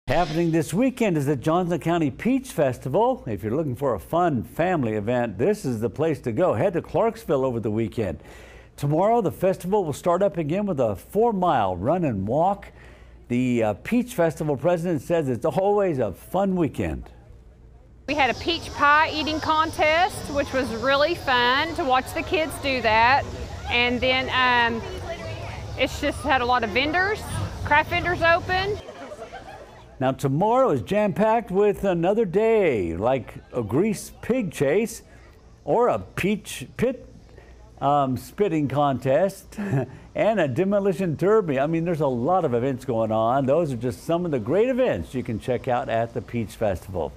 Johnson County celebrates 2019 Peach Festival
The Peach Festival continues this weekend.